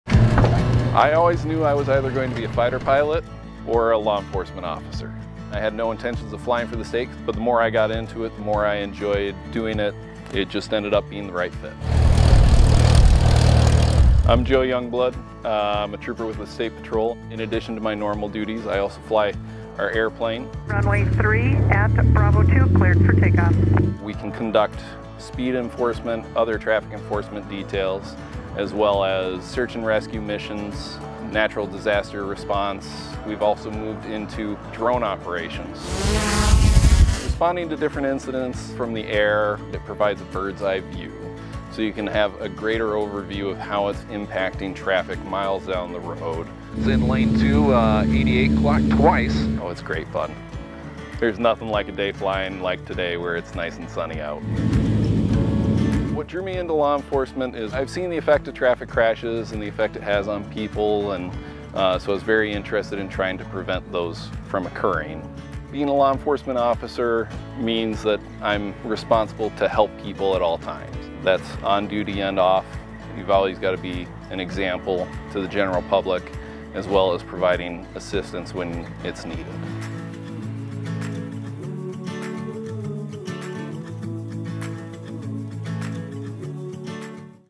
We are Wisconsin State Patrol: Air Support Unit
The Wisconsin State Patrol Air Support Unit provides law enforcement and emergency aerial services to assist partner agencies with public safety across the state. Trooper Youngblood explains why he took his passion to help people to the skies.